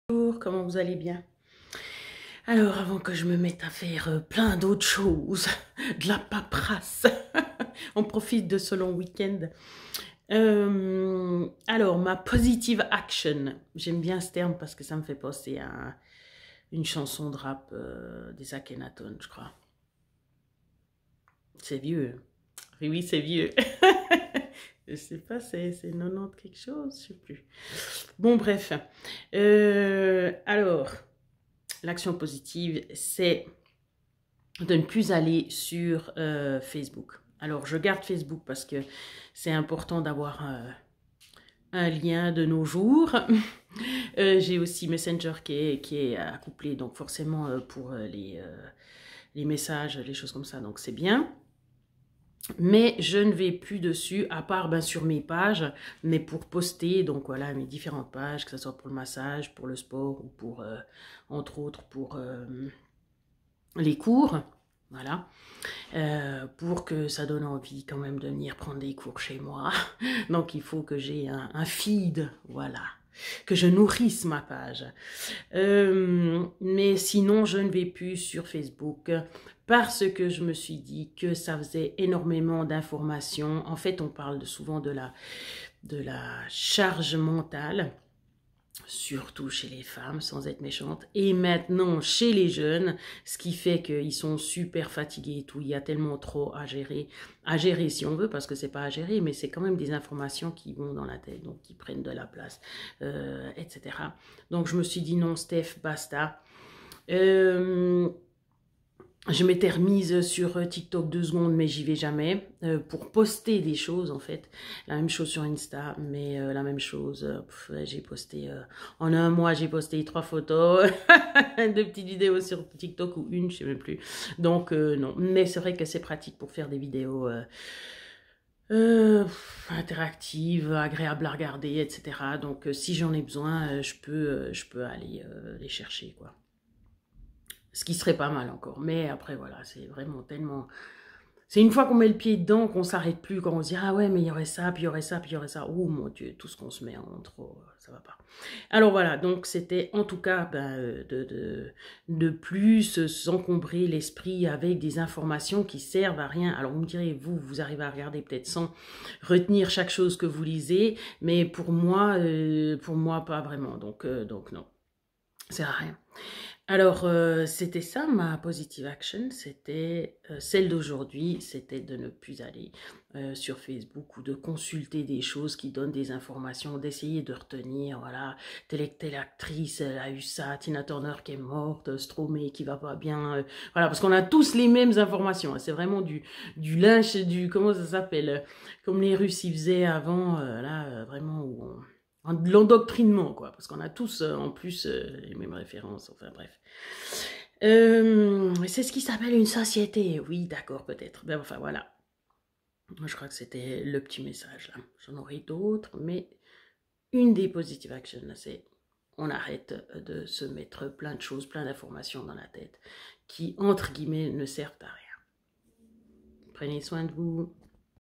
Positiv Action : "arrêter" de consulter les réseaux sociaux
toutes ces informations et stimulations en continu créent une énorme charge mentale, nous fatiguent et nous empêchent de nous concentrer sur l'essentiel, la connexion avec nous-même